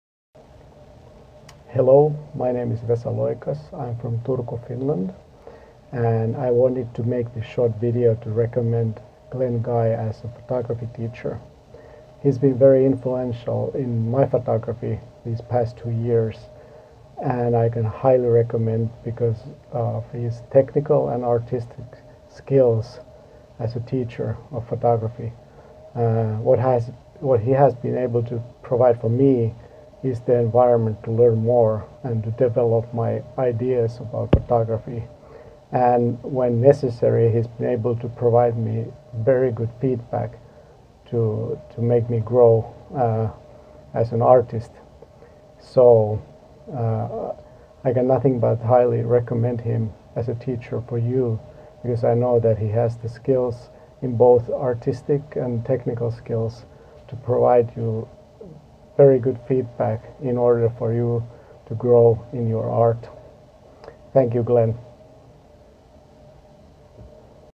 Vesa Loikas testimonial
A testimonial about Glenn Guy, the Travel Photography Guru, from Finnish photographer Vesa Loikas.